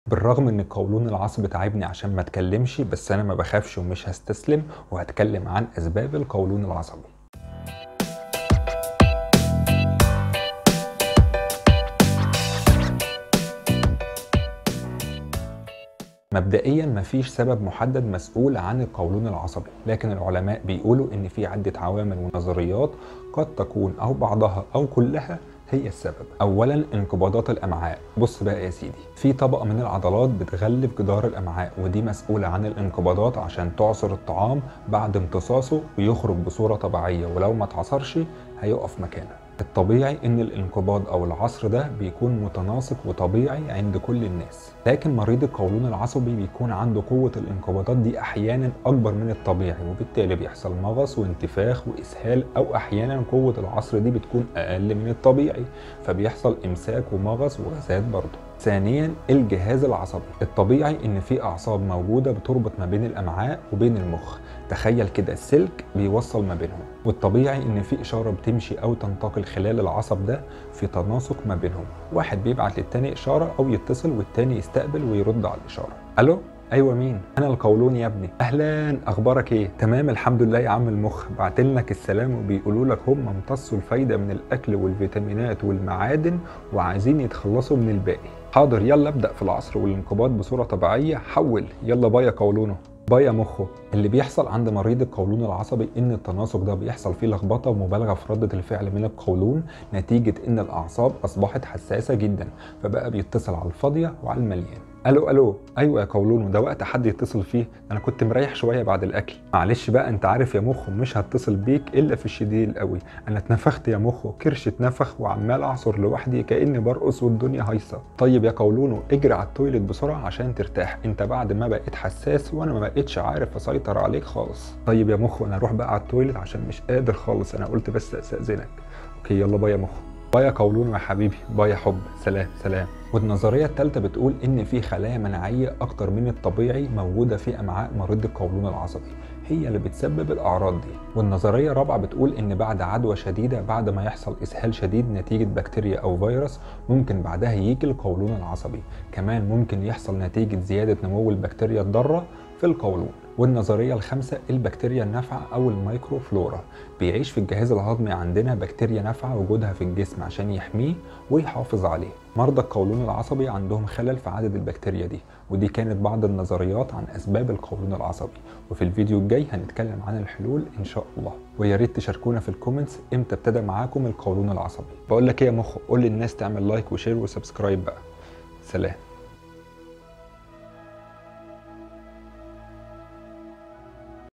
اسباب القولون العصبى
فى الفيديو دا هنتعرف عن اسباب القولون العصبى